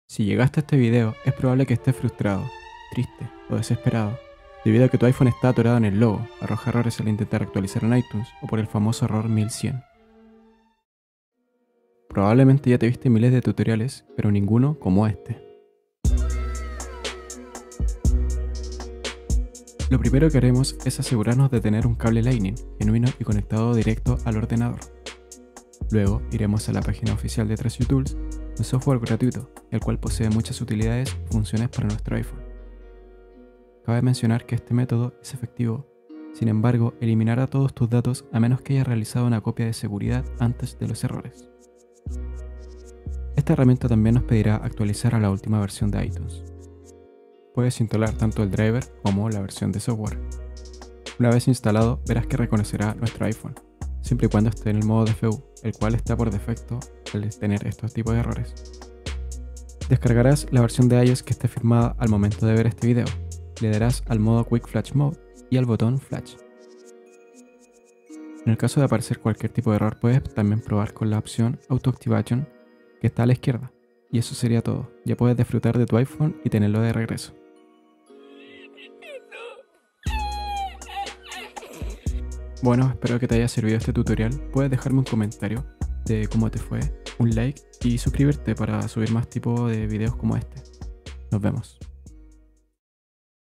[FIX] Solucionar Error al Actualizar iPhone, Restaurar, Código 1100, Boot Loop iTunes - 2023
Tutorial corto de cómo solucionar errores al actualizar, respaldar, restaurar
o alguna duda y un like si te sirvió este método.

0:00 - Inicio
0:19 - Preparación y Herramientas
0:42 - Descarga de iTunes y de iOS
1:03 - Quick Flash Mode
1:22 - Fin

Si quieres más contenidos de este tipo o tutoriales no dudes en decírmelo, como tutoriales de Downgrade de iOS, Jailbreak, Checkra1n, Sideloadly y otras cosas.

Saludos!